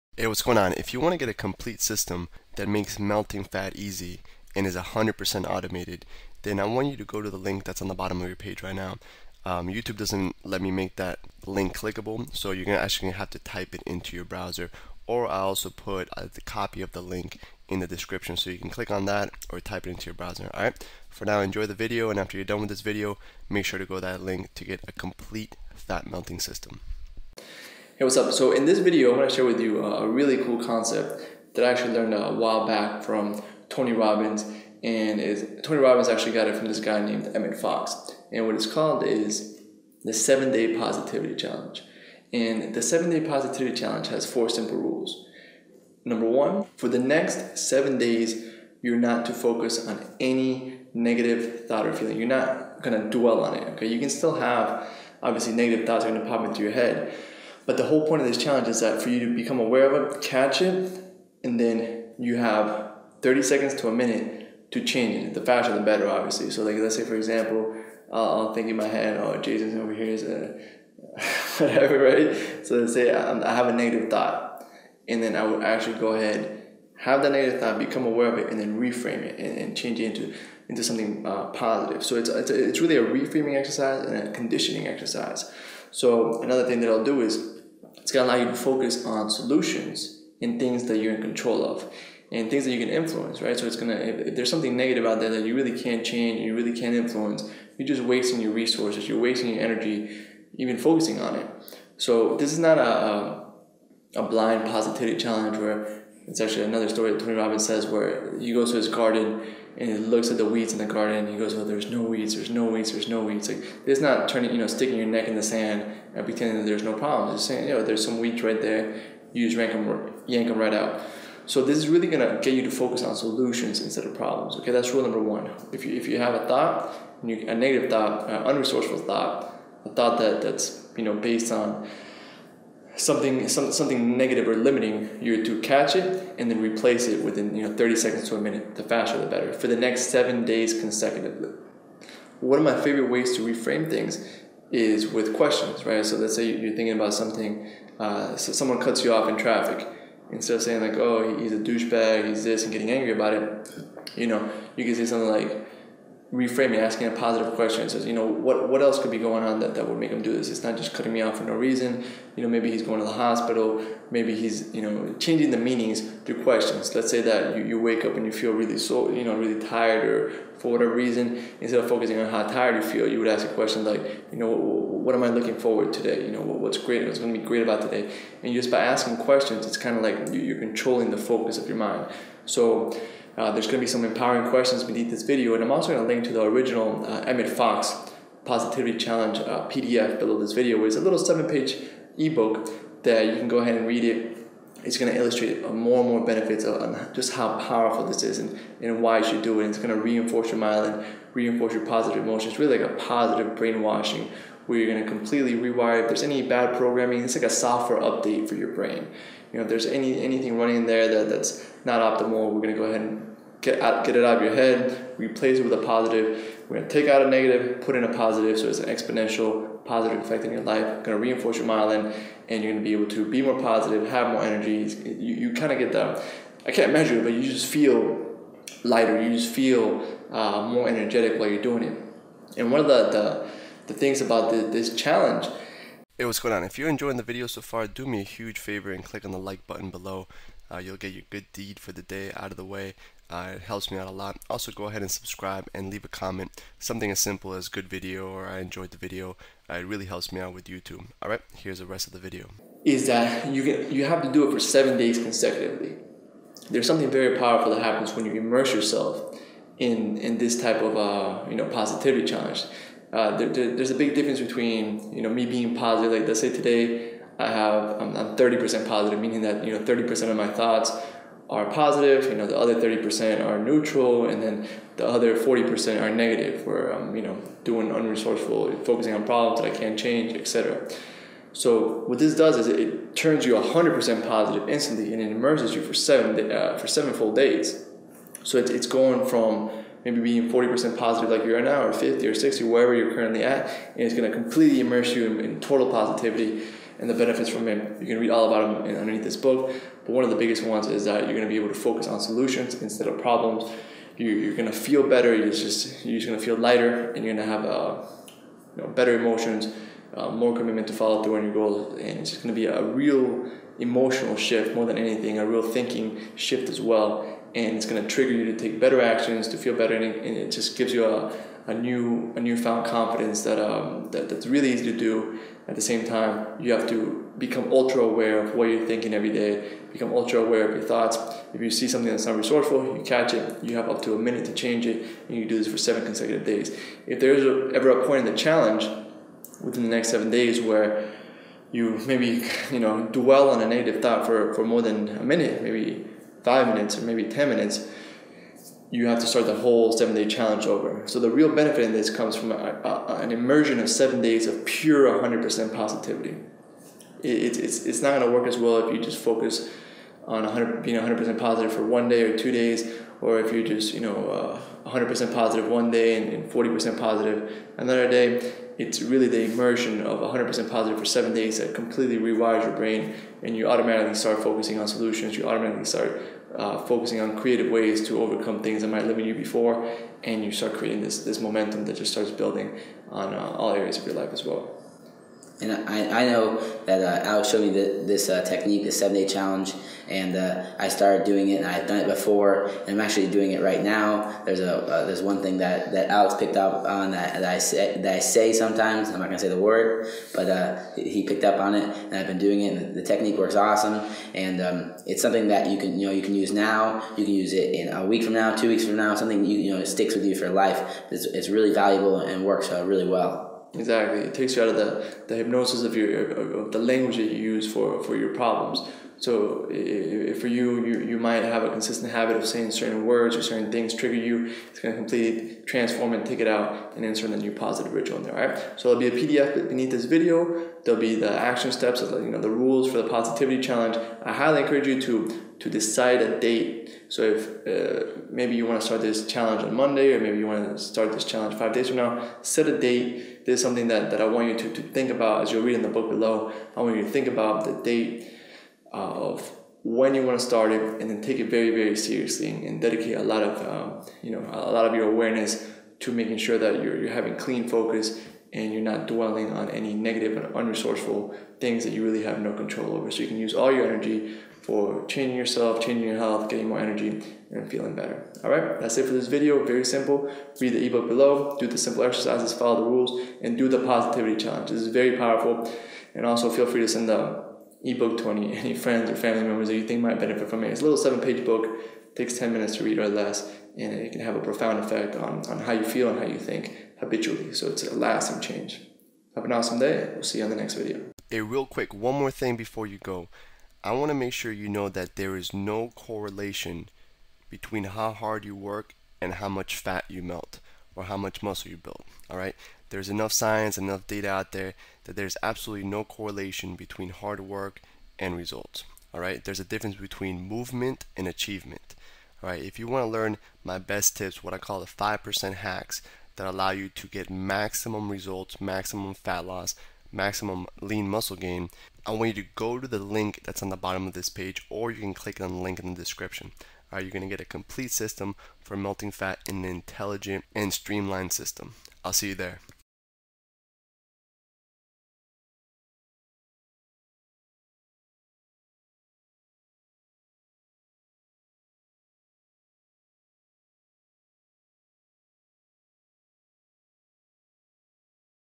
Take Up The Positivity Challenge & Get Your Ideal Weight Now
By building on easy small victories each and every day, you will be igniting your fat loss and boosting your metabolism in no time. Get the secret strategy that keeps your energy and willpower full so that you'll never crash and burn